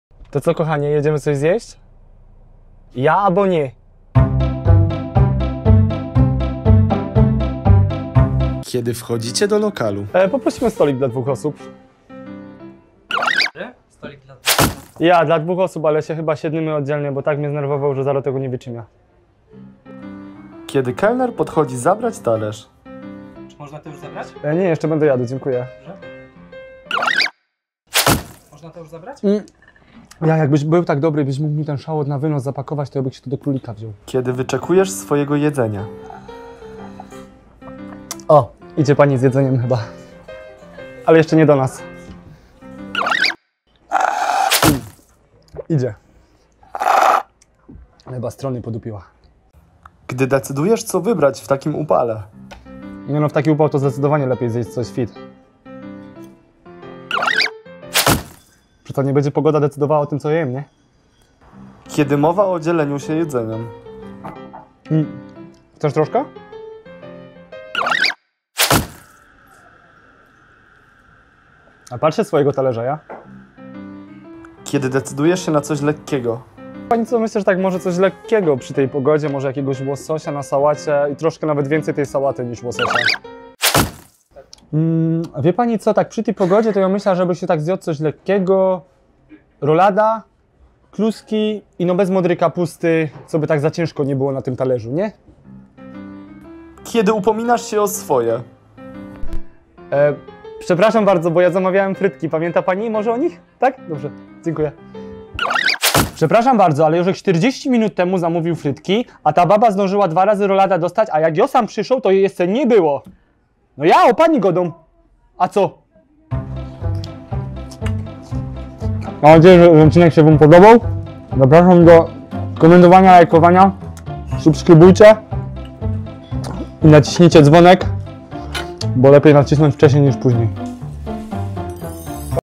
Zryj, rzykej i GODEJ - 7 sytuacji w restauracji (PO ŚLĄSKU)
W restauracji trzeba umieć się zachować. No właśnie, trzeba?
Szczerość to udany przepis na miłość. A miłości daleko niy cza szukać, jak sie idzie zezryć co dobrego.

Video zostało zrealizowane przez UNICORN STUDIOS. Kej chcecie co kamerować na Ślonsku to ino tam!

PS. Dzięki wielkie "W LUFCIE" Bytom za udostępnienie Waszego super ogródka do nagrań ♥️